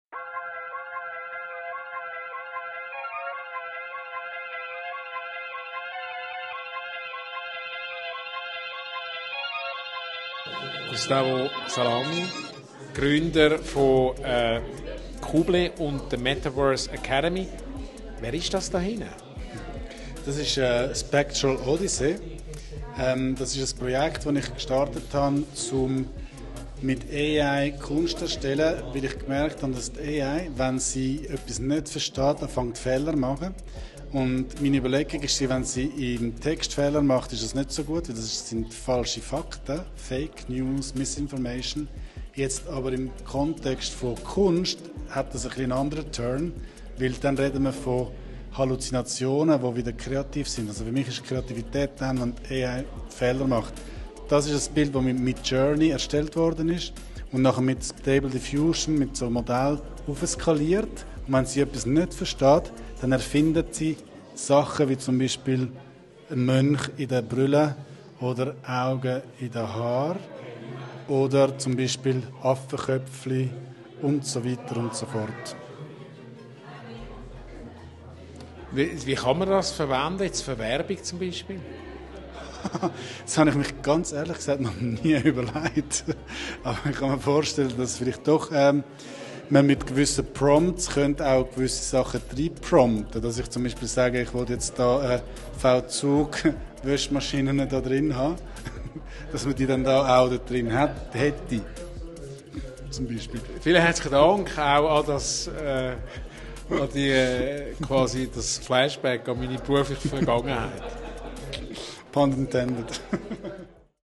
Kuble Gründer Gustavo Salami präsentiert sein KI-Kunstprojekt «Spectral Odyssey»
Gustavo Salami, Gründer von Kuble und der Metaverse Academy, spricht über sein Herzens-Projekt Spectral Odyssey, bei dem er mithilfe von Künstlicher Intelligenz (KI) Kunst kreiert hat. Die KI macht bei Unverständnis Fehler, sogenannte Halluzinationen. Was im Arbeitsumfeld unerwünscht ist, wurde in diesem Projekt bewusst gefördert. Für Salami ist Kreativität in der KI das Ergebnis von Fehlern. In mit Midjourney und Stable Diffusion entwickelten Bildern wird die KI «kreativ» und fügt eigenständig ungewöhnliche und überraschende Bildelemente hinzu. Zu experimentieren und Tools auszureizen findet Salami wichtig, um am Ball zu bleiben und die Erfahrungen für die Weiterbildung von Mitarbeitenden, Teams und für Unternehmen mitzunehmen.